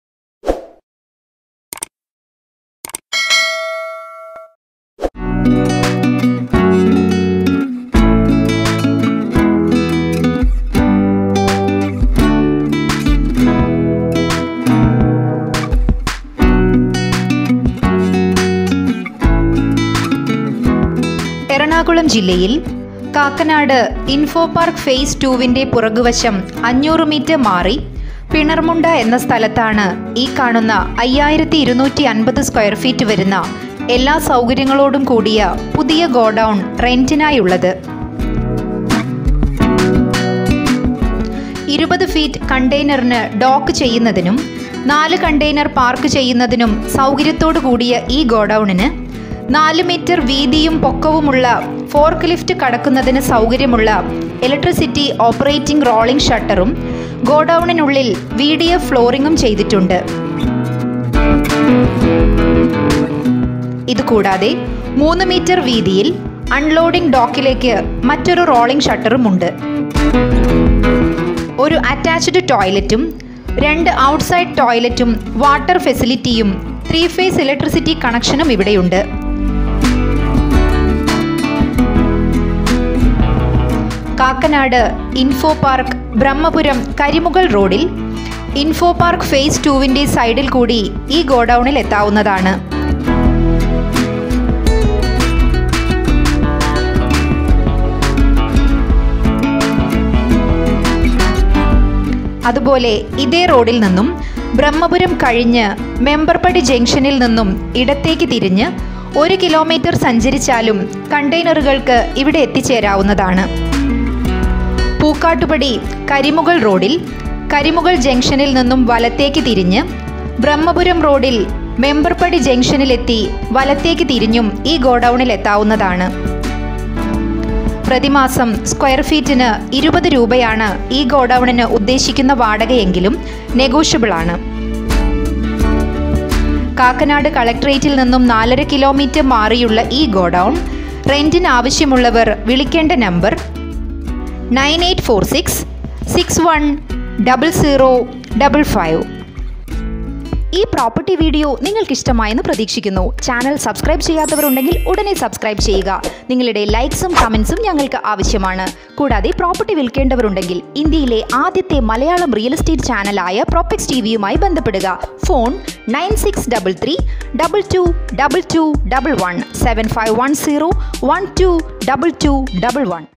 godown for rent in Ernakulam|Commercial properties in Ernakulam|SHEMEER EKM 28 11 2021 UNNI
Ernakulam district Kakkanad infopark phase 2 backside 500 meter away Pinarmunda place this 5250 sqft all facilities new godown situated.
20 meter container docking capacity and 4 container parking facility and 4 meter width and height to enter folk lift electrically operating rolling shutter and vdf flooring in godown.
another unloding rock lift shutter with 3m.
1 attached toilet,2 outside toilet facility and water facility and three phase electricity.
A way to reach this godown is kakkanad infopark brahmapuram karimukal road infopark phase 2 side and another way to reach containers is same road after Brahmapuram left 1 km away to enter this godown.
Pukkattupadi Karimukal road karimukal junction turn right Brahmapuram road Memberpadi junction right 1 km away to reach this godown.
rent;20/sqft/mont,Negotiable
4.5 km away from Kakkanad Collectorate.